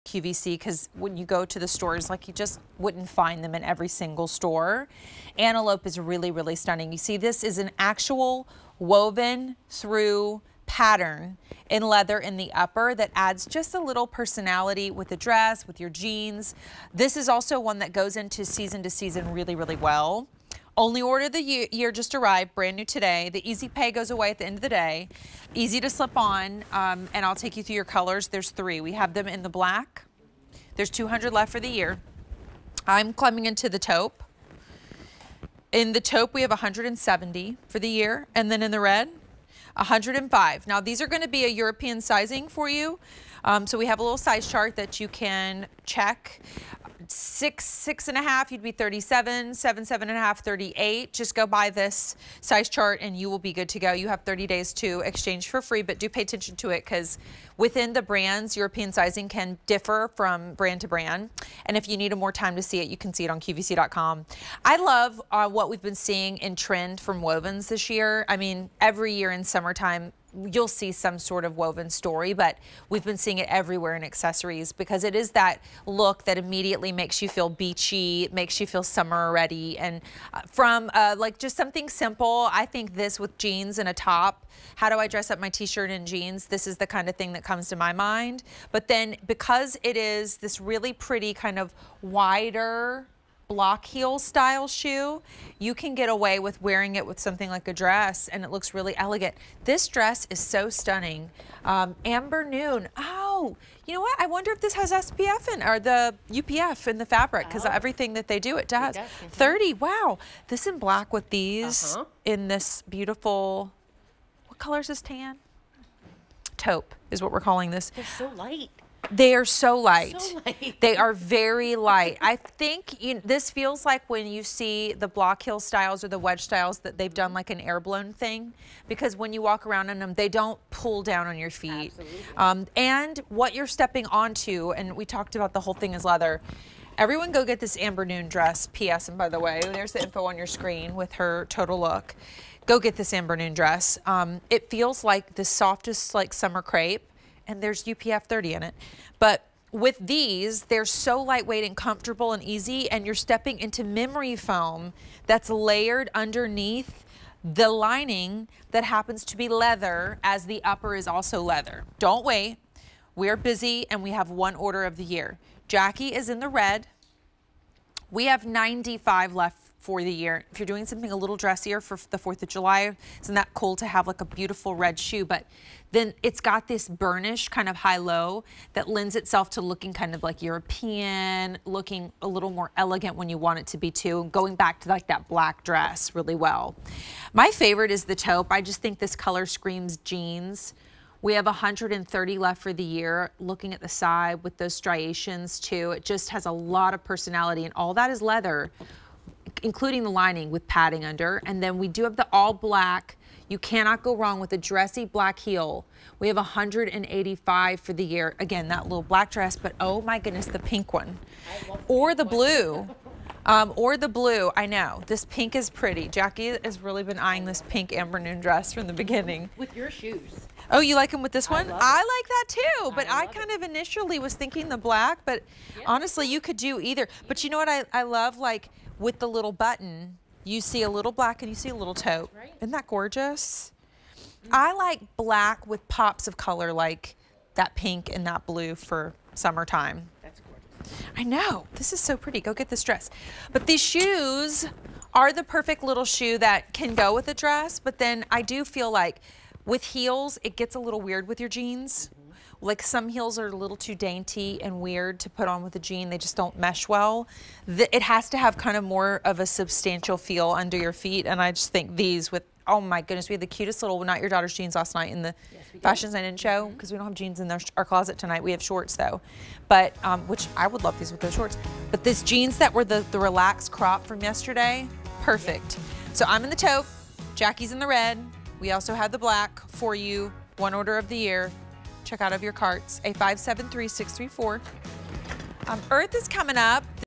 Antelope Leather Woven Heeled Sandals - Tamia on QVC
Antelope Leather Woven Heeled Sandals - Tamia

How chic is the Tania? The lightweight clog gleams with a stylish fashion that your favorite sunny-season OOTDs will love. From Antelope.